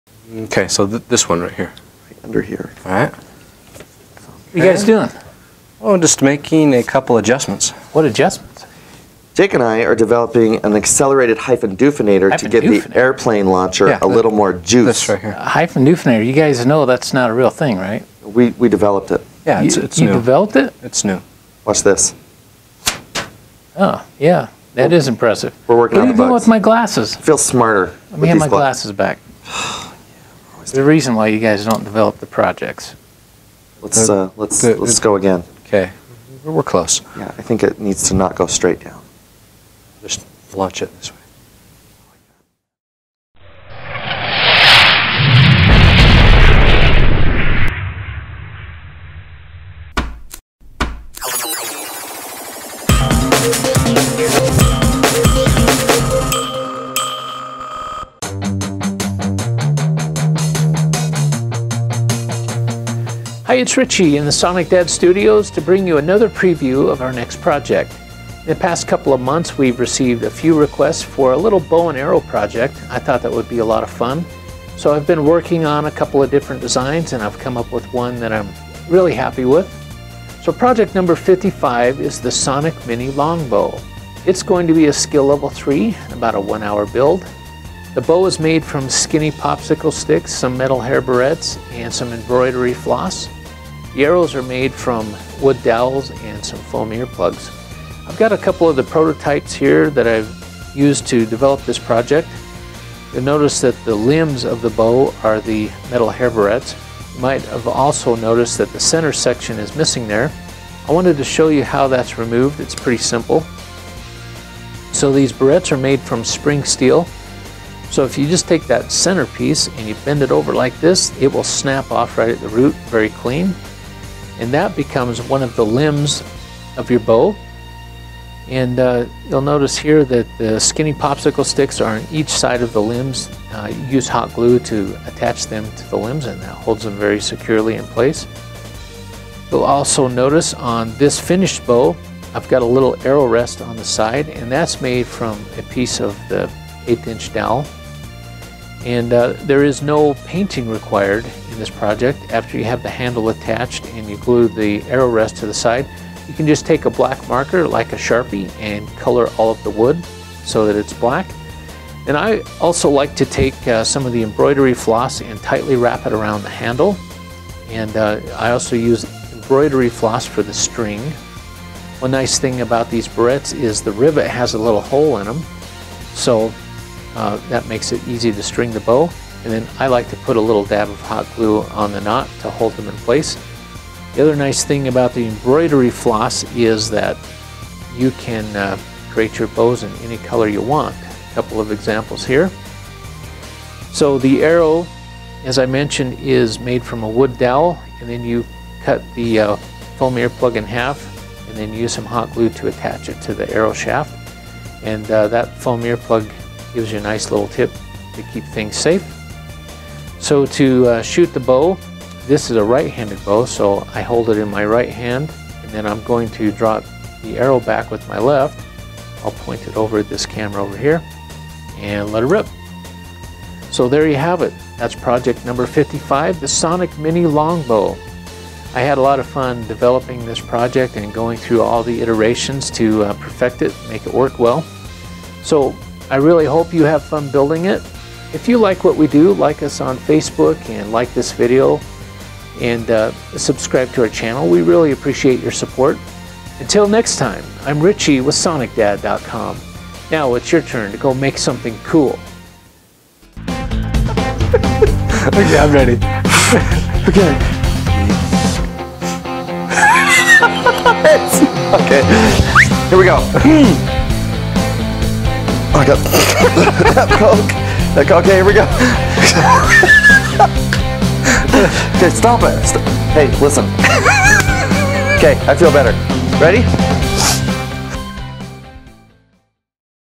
Area 51 1/2 Sneak Preview: SonicDad project #55 - The Sonic Mini Long Bow and Arrow
Introducing the introduction of SonicDad Project #55: The Sonic Mini Longbow. There are lots of ways to make a mini bow and arrow. We'll show you how to make one out of spring steel. We've also added a few little secrets to our videos. We hope you enjoy.